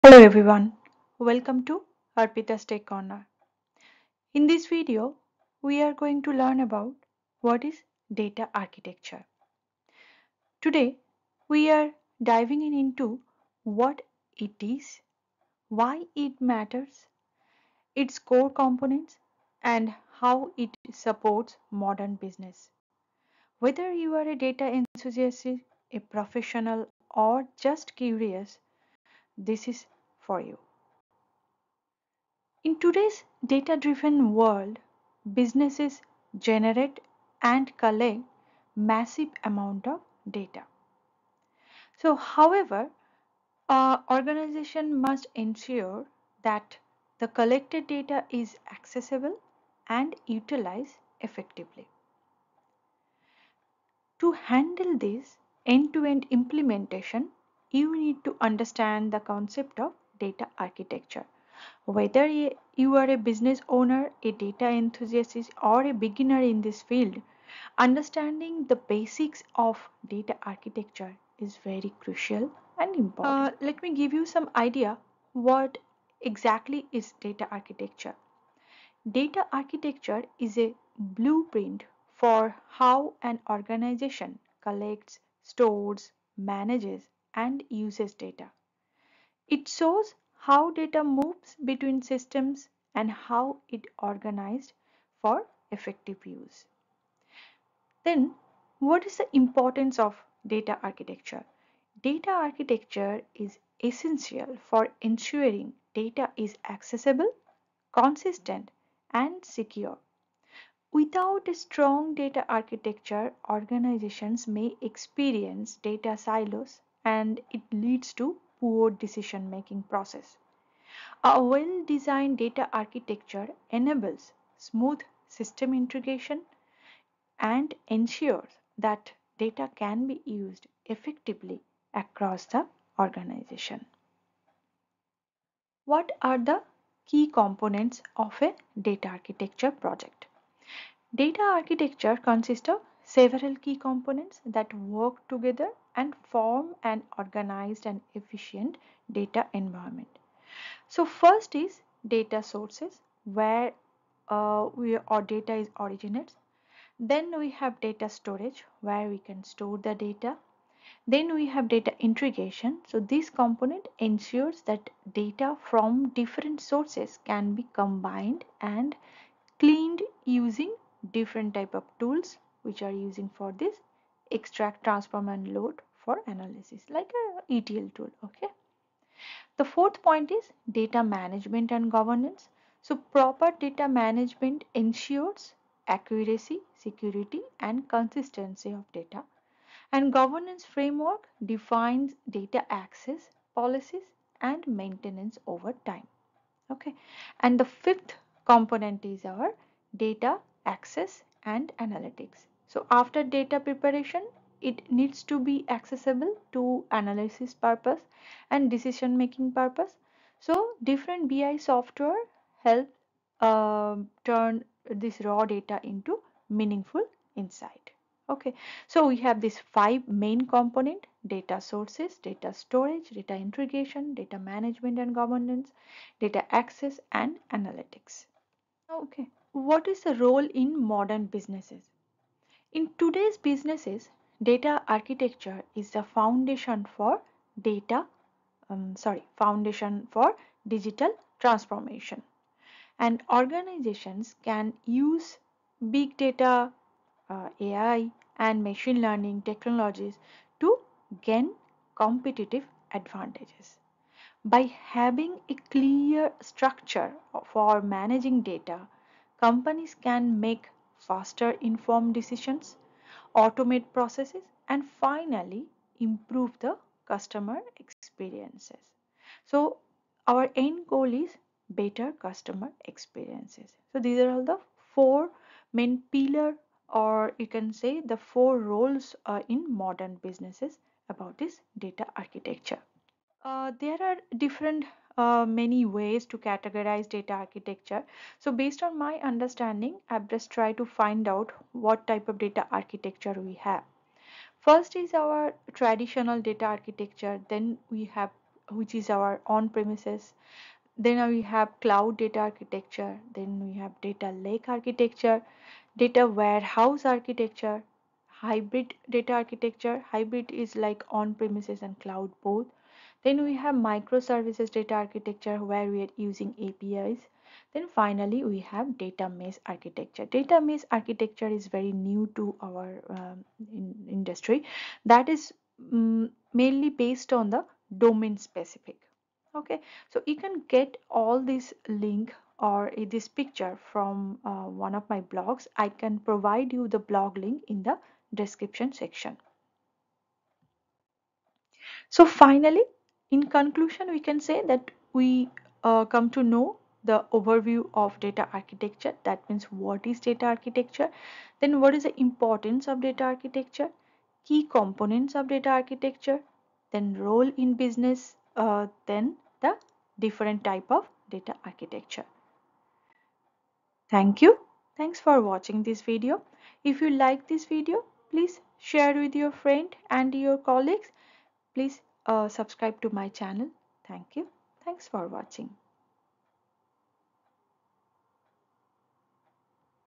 Introduction to Data Architecture | Data Architecture Concept | Chap 1
Welcome to our in-depth video on Data Architecture —the backbone of modern data strategy!
1. overview and importance of data architecture
2. key components
3. types of data architecture,
4. the role it plays in empowering businesses

Whether you’re a data professional, tech enthusiast, or just curious about how companies handle and leverage data, this video will discuss the essentials.